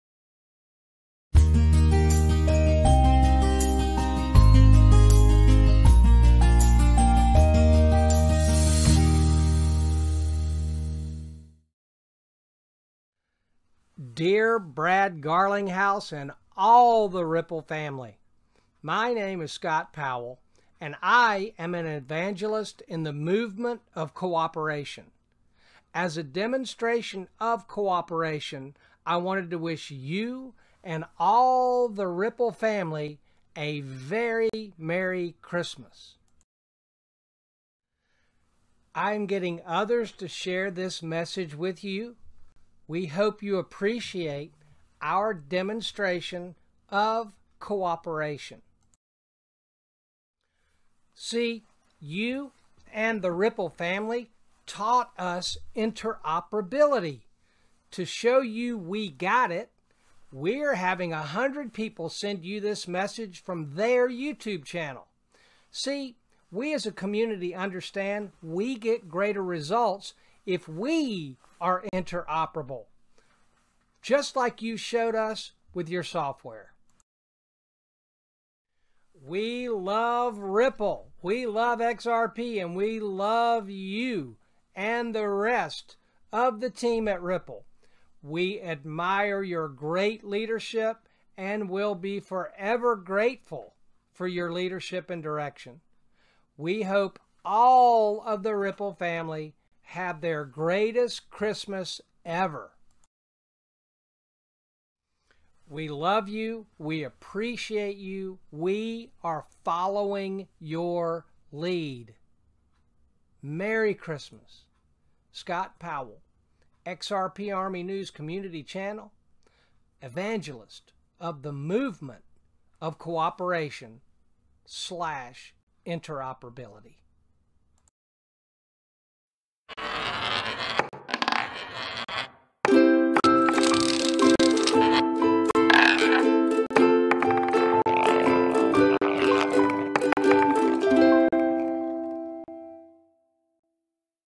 RIPPLE/XRP XRP COMMUNITY CHRISTMAS MESSAGE BRAD GARLINGHOUSE 315 Montgomery St San Francisco CA 94
Dear Brad Garlinghouse and all the Ripple Family!

My name is Scott Powell and I am an Evangelist in the Movement of Cooperation! As a Demonstration of Cooperation I wanted to Wish you and all The Ripple Family a Very Merry Christmas.

Getting others to share this message with you….hope you appreciate our Demonstration of Cooperation.

See you and the Ripple Family taught us INTEROPERABILITY….to show you WE GOT IT….we are having 100 people send you this message from their YouTube Channel. See we as a Community understand we get greater results if WE ARE INTEROPERABLE…..but like you showed us with your software.

We love Ripple, we love Xrp and we love and and the rest of the Team at Ripple.

We admire your Great Leadership and will be forever grateful for your Leadership.

We hope all of the Ripple Family have the greatest Christmas ever.

We love you, we appreciate you, we are following your Lead.

Scott Powell
XRP ARMY NEWS COMMUNITY CHANNEL
Evangelist of the Movement of Cooperation(interoperability)